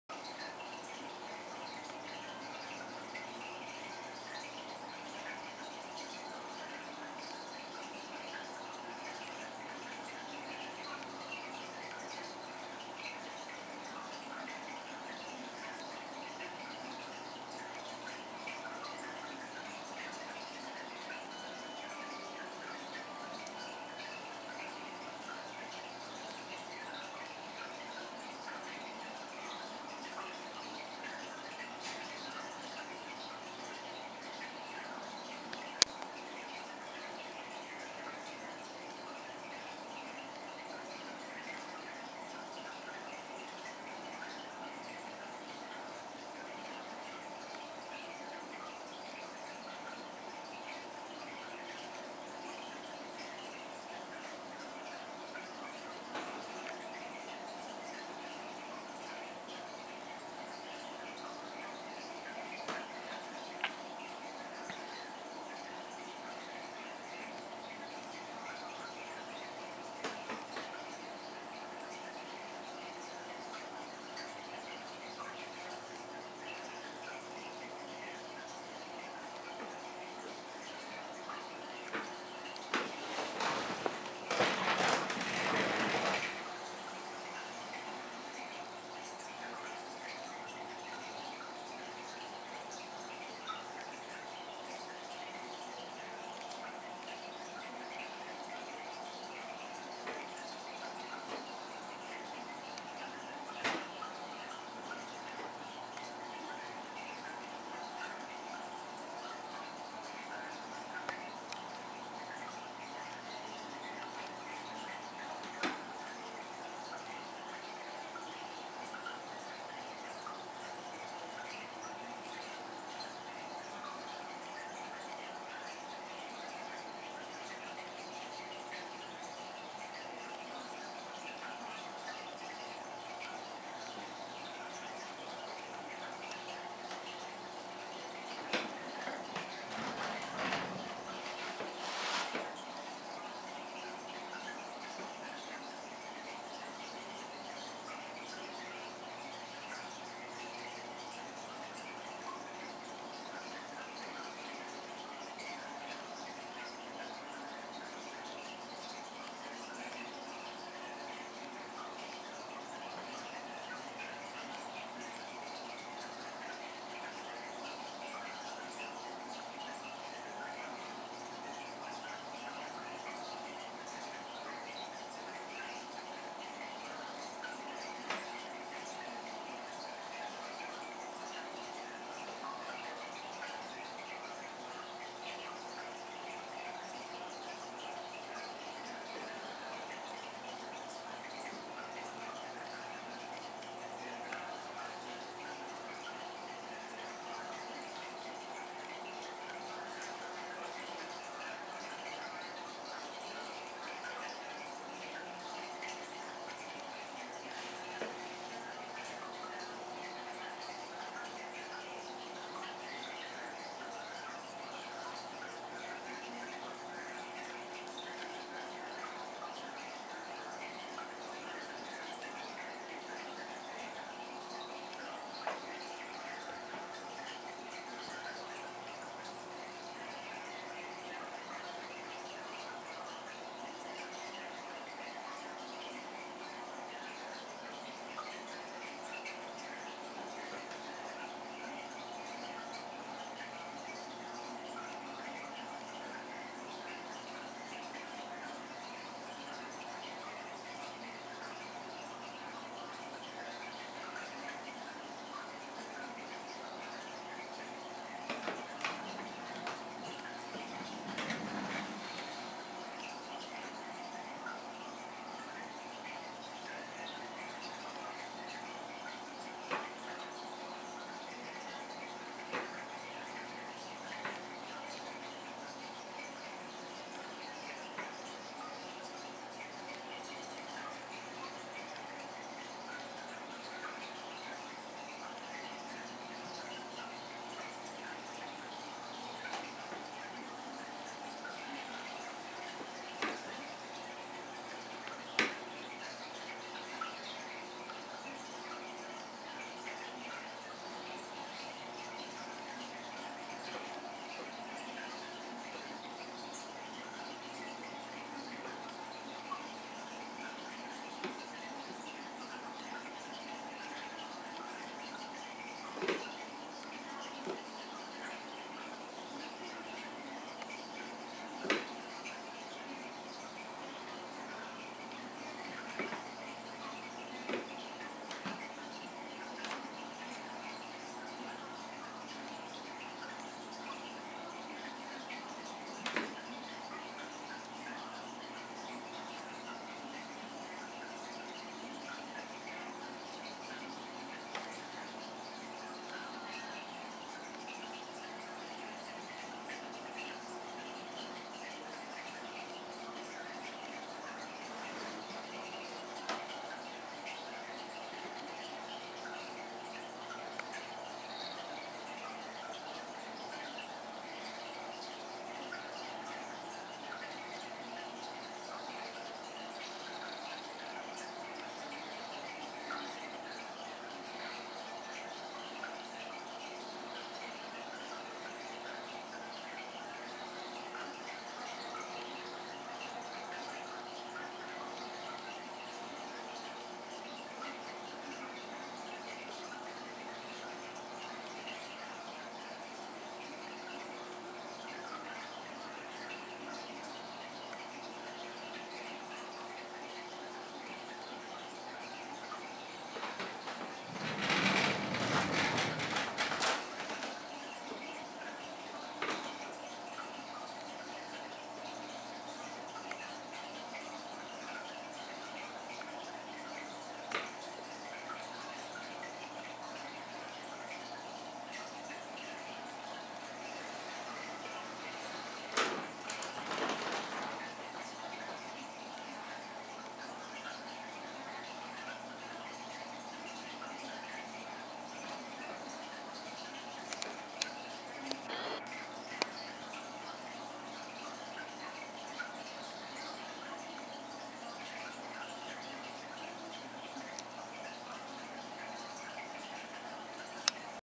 "TANK" featuring "BOWLING BALL HEAD THE ENFORCER"
Bowling Ball head is on of two convicts in a 75 gallon tank of ciclids.
BBH is so agressive that his lips stay chapped,
Most of his attention is focused on the othe convict in the tank.